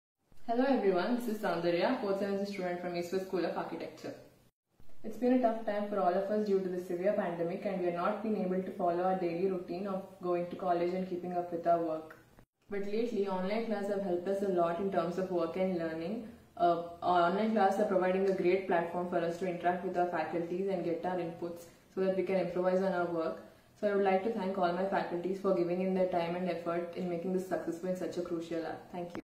Corona(COVID19) Lockdown Experience Sharing
EWSA Student Sharing his Experience about the college and faculty during Corona(COVID19) Lockdown.